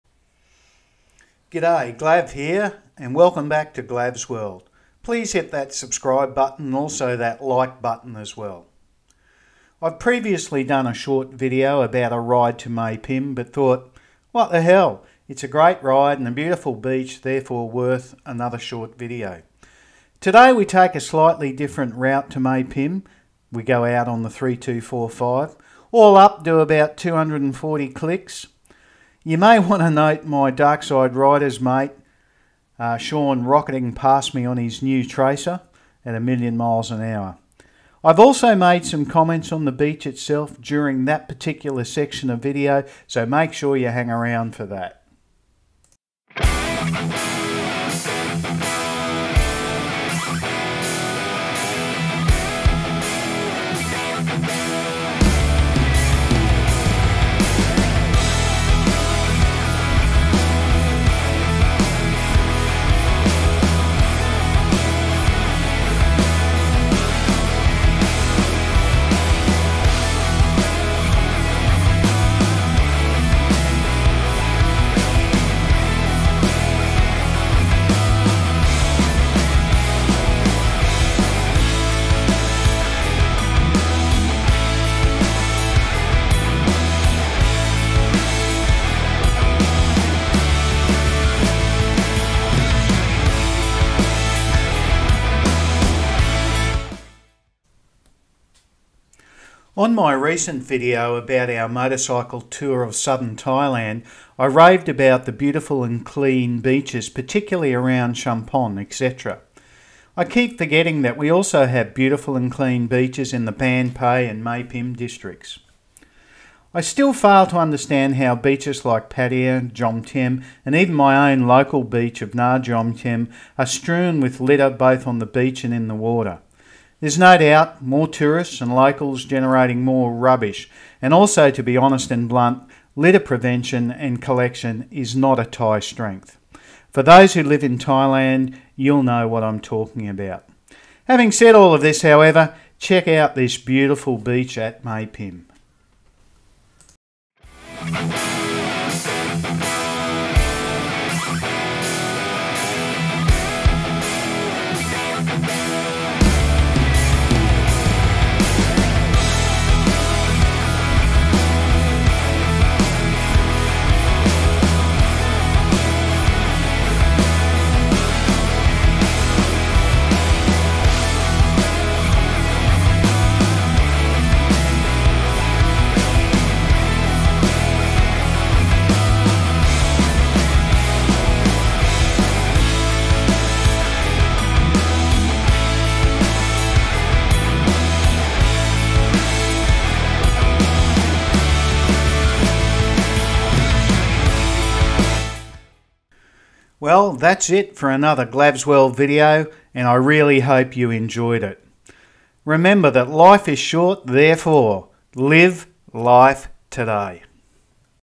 Glav's World Video - Motorcycle Ride from NAJomtien to MaePhim
Check out this latest Motorcycle Ride from NaJomtien to Mae Phim. Mae Phim is a beautiful beach and its a really nice scenic relaxed ride to get their.